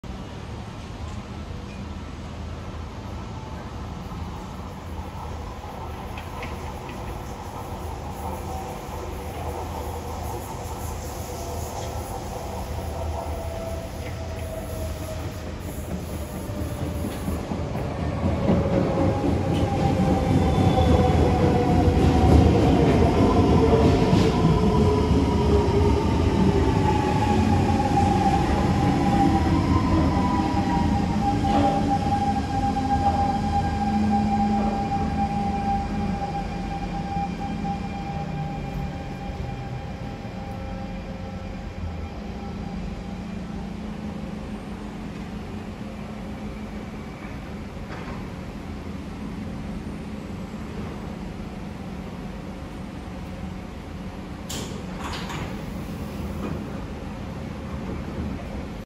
[Zero][SMRT] C651 (207/208) arriving Ang Mo Kio(5)
-Train Details
C651 (207/208)
Build By: Siemens Aktiengesellschaft & SGP Verkehrstechnik
Year Build: 1994
Year Entered Service: 1995
Year Refurbished: N/A
Motor: GTO-VVVF (Siemens)
TRN: 226

With 213/214 being transferred out of Ulu Pandan Depot(UPD) under T901 on 02/11/2023 and 207/208 deployed under T113 out of UPD on 03/11/2023, there are currently zero C651 trains left in the North South Line(NSL).

207/208 is 1 of the remaining 6 C651 trains left.